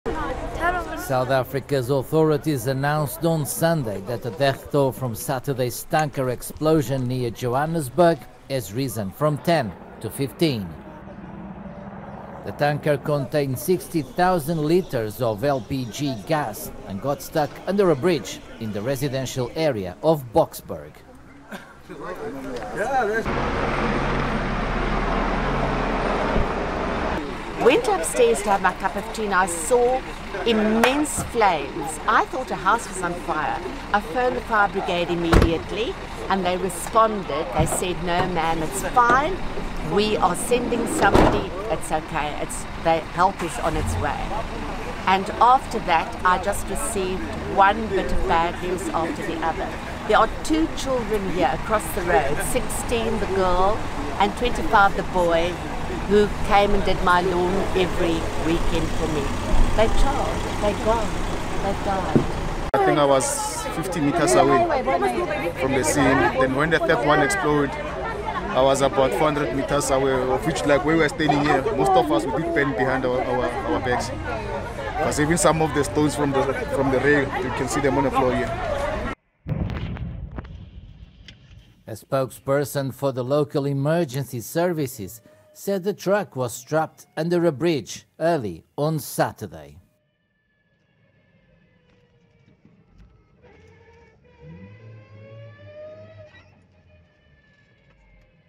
Death toll from tanker explosion in South Africa climbs to 15
The tanker carried 60,000 litres of LPG gas and got stuck under a bridge near Johannesburg on Saturday morning